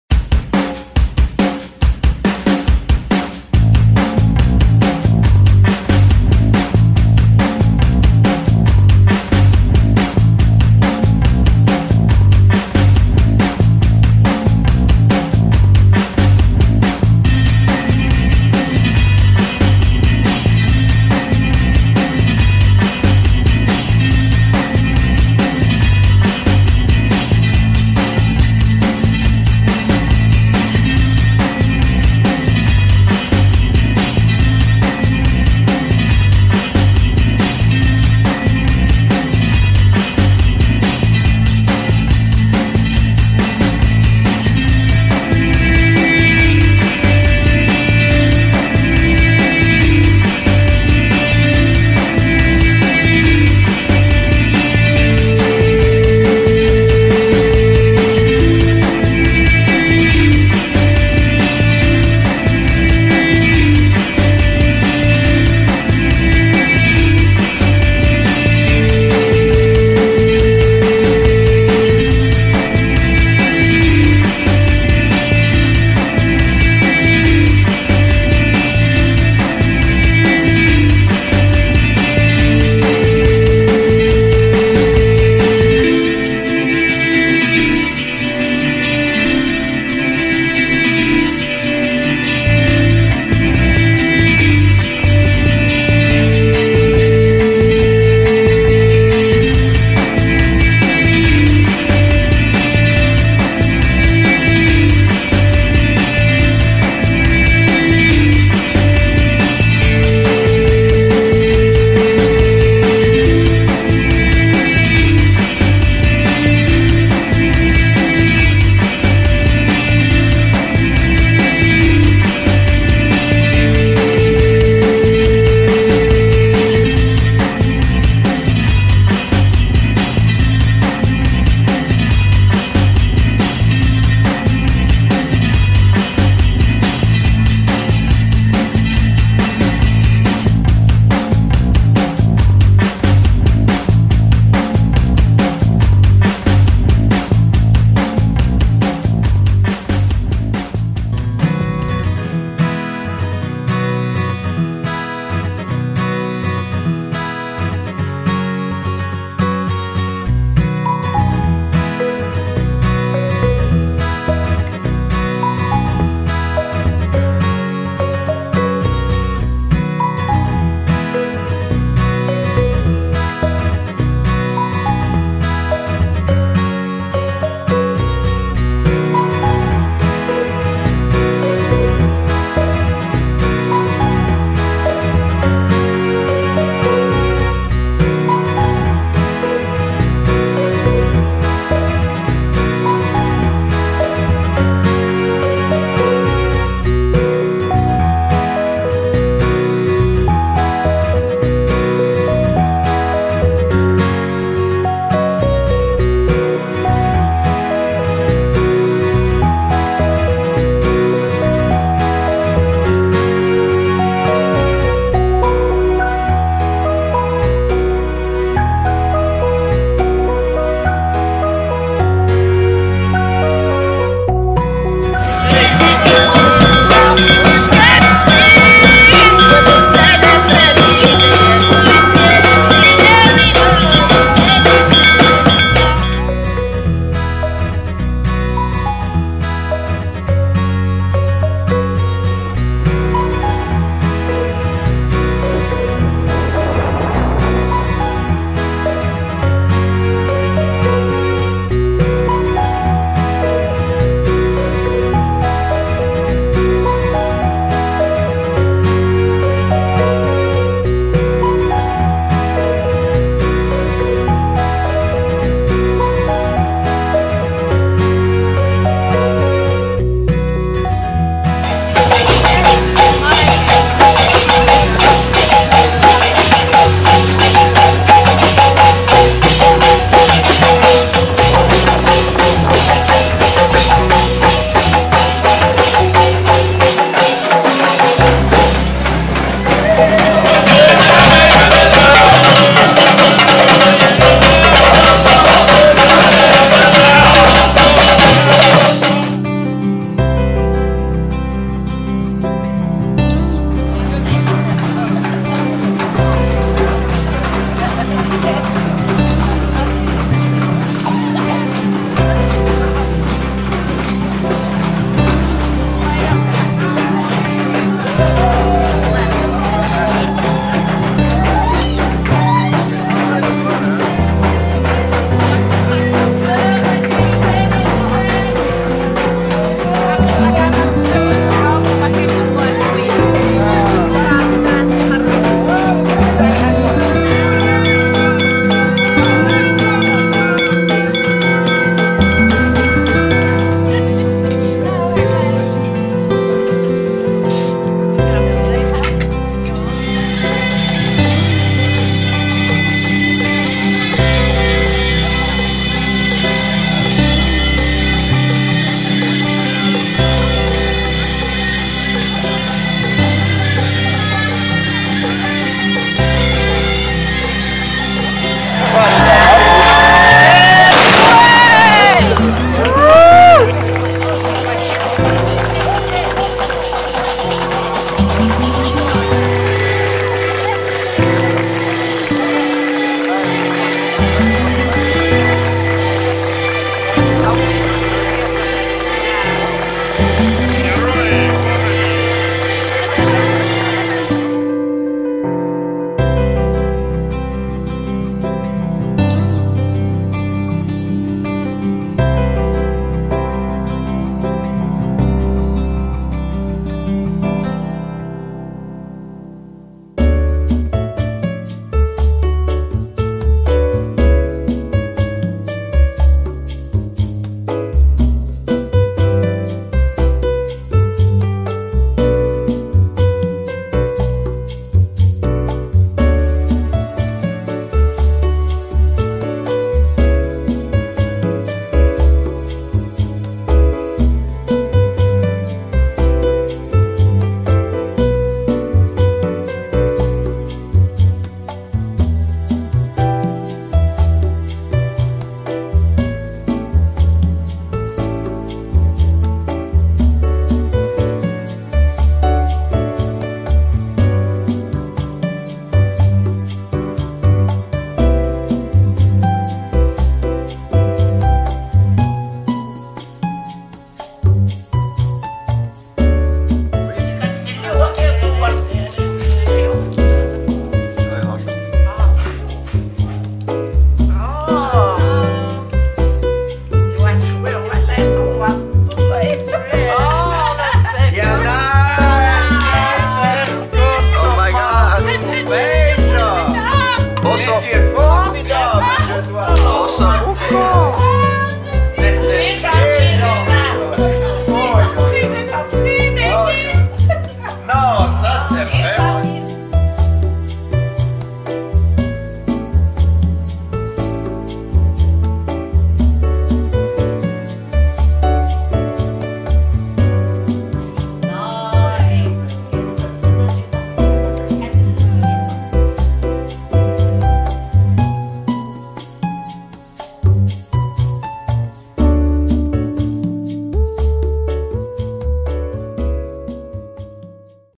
2018 Marrakech
Vacation in April/May 2018  in Marrakech, Morocco, with our friends Hans and Farida.